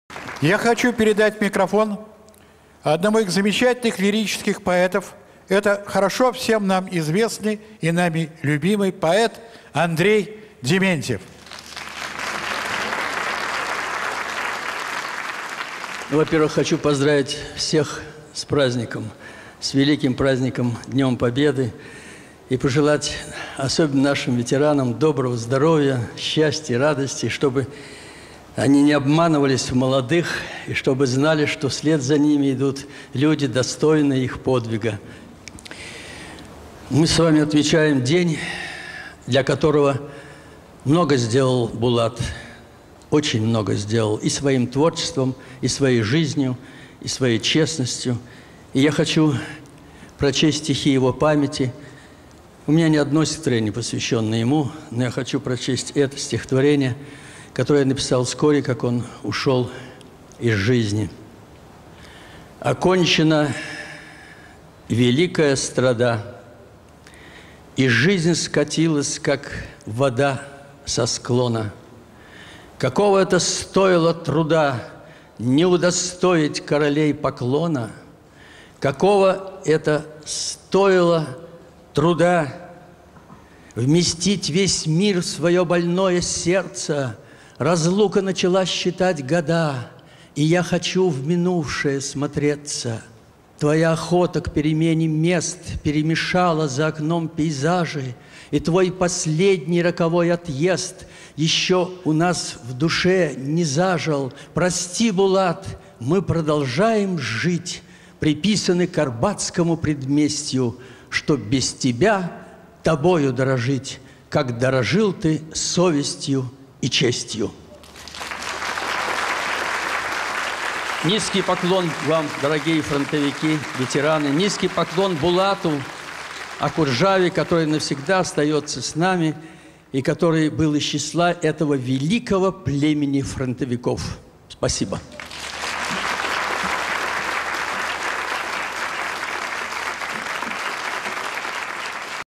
Андрей Дементьев - Памяти Булата Окуджавы.
Фрагмент концерта в доме-музее Булата Окуджавы в Переделкино 2010 г.